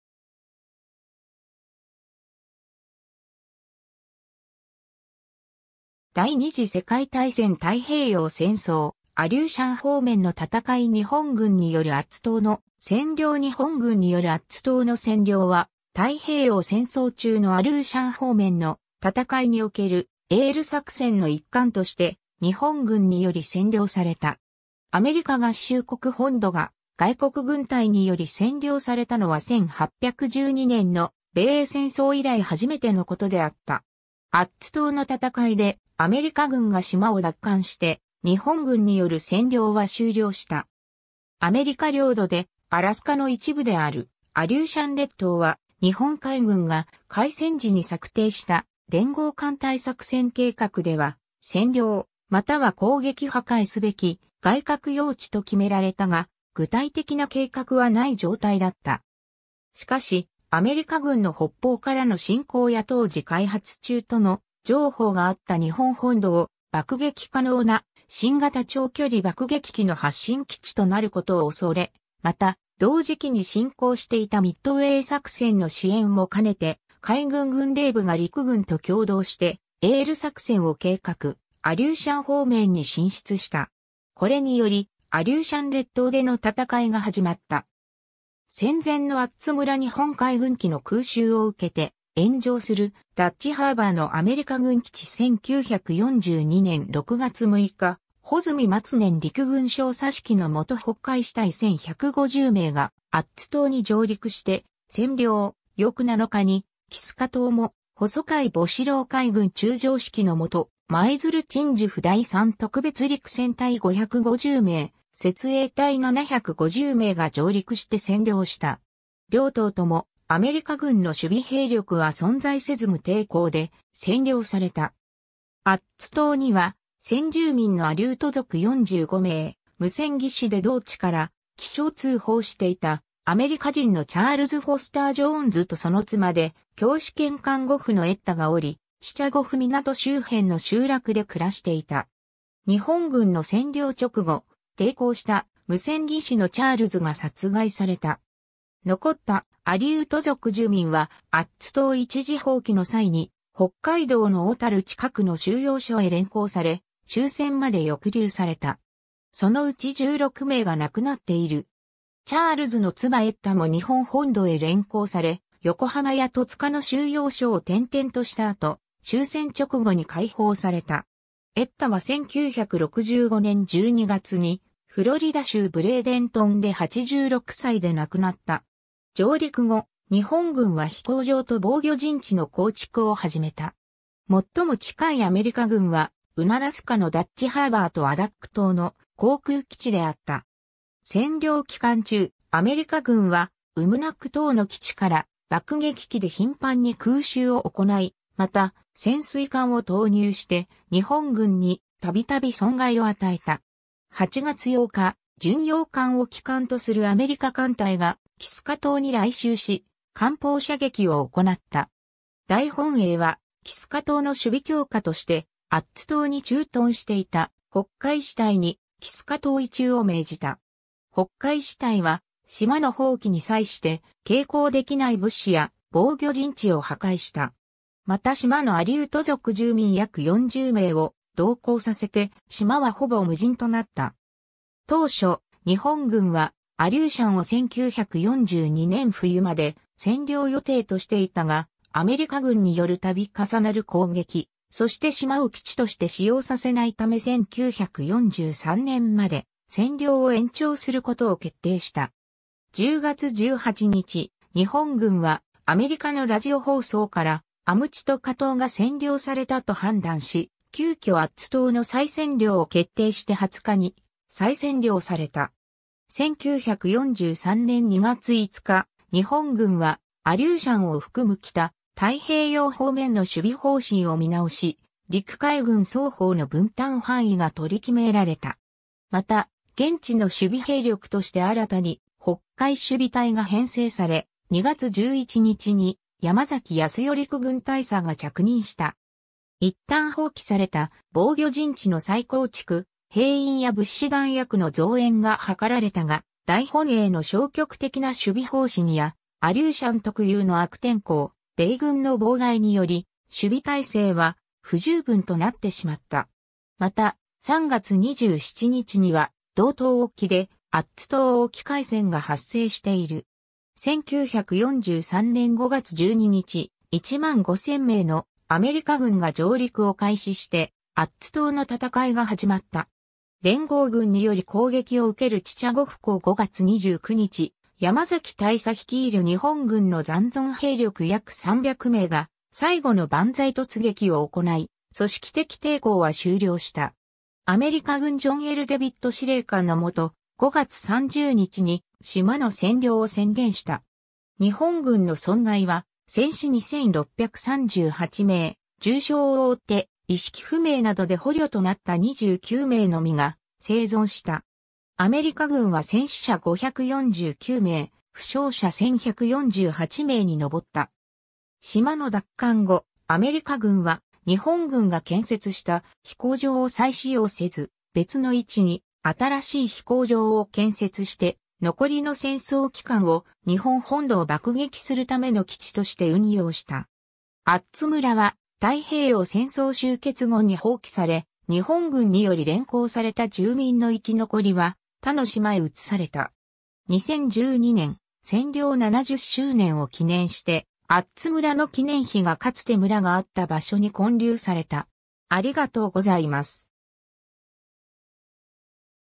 日本軍によるアッツ島の占領
第二次世界大戦 」 太平洋戦争 」 アリューシャン方面の戦い 」 日本軍によるアッツ島の占領 日本軍によるアッツ島の占領は、太平洋戦争中のアリューシャン方面の戦いにおけるAL作戦の一環として、日本軍により占領された。
アメリカ合衆国本土が外国軍隊により占領されたのは1812年の米英戦争以来初めてのことであった。
アッツ島の戦いでアメリカ軍が島を奪還して日本軍による占領は終了した。
 アメリカ領土でアラスカの一部であるアリューシャン列島は、日本海軍が開戦時に策定した連合艦隊作戦計画では「占領または攻撃破壊すべき外郭要地」と決められたが、具体的な計画は無い状態だった。
 しかし、アメリカ軍の北方からの進攻や当時開発中との情報があった日本本土を爆撃可能な新型長距離爆撃機の発進基地となることを恐れ、また同時期に進行していたミッドウェー作戦の支援も兼ねて、海軍軍令部が陸軍と共同してAL作戦を計画、アリューシャン方面に進出した。
これによりアリューシャン列島での戦いが始まった。
 戦前のアッツ村 日本海軍機の空襲を受けて炎上するダッチハーバーのアメリカ軍基地 1942年6月6日、穂積松年陸軍少佐指揮のもと北海支隊1,150名がアッツ島に上陸して占領、翌7日にキスカ島も細萱戊子郎海軍中将指揮の下、舞鶴鎮守府第三特別陸戦隊550名、設営隊750名が上陸して占領した。
両島ともアメリカ軍の守備兵力は存在せず無抵抗で占領された。
アッツ島には先住民のアリュート族45名、無線技士で同地から気象通報していたアメリカ人のチャールズ・フォスター・ジョーンズとその妻で教師兼看護婦のエッタがおり、チチャゴフ港周辺の集落で暮らしていた。
 日本軍の占領直後、抵抗した無線技師のチャールズが殺害された。
残ったアリュート族住民はアッツ島一時放棄の際に北海道の小樽近くの収容所へ連行され、終戦まで抑留された。
そのうち16名が亡くなっている。
チャールズの妻エッタも日本本土へ連行され、横浜や戸塚の収容所を転々とした後、終戦直後に解放された。
エッタは1965年12月にフロリダ州ブレイデントンで86歳で亡くなった。
 上陸後、日本軍は飛行場と防御陣地の構築を始めた。
最も近いアメリカ軍はウナラスカのダッチハーバーとアダック島の航空基地であった。
占領期間中、アメリカ軍はウムナック島の基地から爆撃機で頻繁に空襲を行い、また潜水艦を投入して日本軍に度々損害を与えた。
 8月8日、巡洋艦を基幹とするアメリカ艦隊がキスカ島に来襲し、艦砲射撃を行った。
大本営はキスカ島の守備強化としてアッツ島に駐屯していた北海支隊にキスカ島移駐を命じた。
北海支隊は島の放棄に際して、携行できない物資や防御陣地を破壊した。
また島のアリュート族住民約40名を同行させて島はほぼ無人となった。
 当初、日本軍はアリューシャンを1942年冬まで占領予定としていたが、アメリカ軍によるたび重なる攻撃、そして島を基地として使用させないため1943年まで占領を延長する事を決定した。
10月18日、日本軍はアメリカのラジオ放送からアムチトカ島が占領されたと判断し、急遽アッツ島の再占領を決定して20日に再占領された。
 1943年2月5日、日本軍はアリューシャンを含む北太平洋方面の守備方針を見直し、陸海軍双方の分担範囲が取り決められた。
また、現地の守備兵力として新たに北海守備隊が編成され、2月11日に山崎保代陸軍大佐が着任した。
いったん放棄された防御陣地の再構築、兵員や物資弾薬の増援が図られたが、大本営の消極的な守備方針やアリューシャン特有の悪天候、米軍の妨害により守備体制は不十分となってしまった。
 また、3月27日には同島沖でアッツ島沖海戦が発生している。
 1943年5月12日、15,000名のアメリカ軍が上陸を開始してアッツ島の戦いが始まった。
 連合軍により攻撃を受けるチチャゴフ港 5月29日、山崎大佐率いる日本軍の残存兵力約300名が最後のバンザイ突撃を行い、組織的抵抗は終了した。
アメリカ軍ジョン･L･デヴィット司令官の下、5月30日に島の占領を宣言した。
日本軍の損害は戦死2,638名、重傷を負って意識不明などで捕虜となった29名のみが生存した(生存率は約1%)。
アメリカ軍は戦死者549名、負傷者1,148名に上った。
島の奪還後、アメリカ軍は日本軍が...